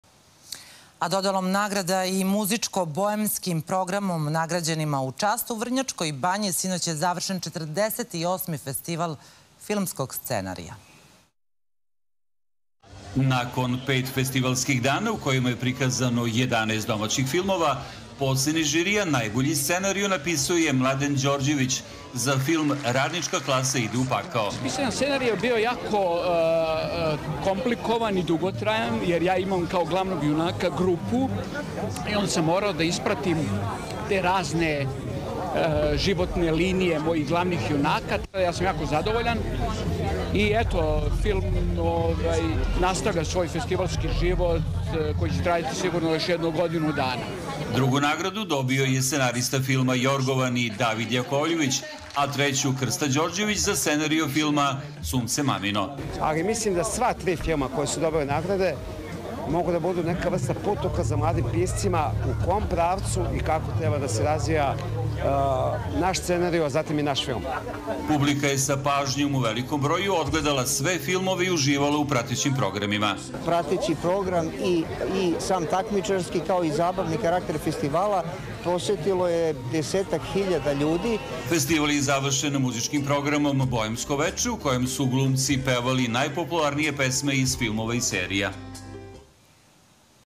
Prilog B92 – 18.08.2024.
Dodelom nagrada i muzičko-boemskim programom “Nagrađenima u čast” završen je 48. Festival filmskog scenarija, prenosi B92.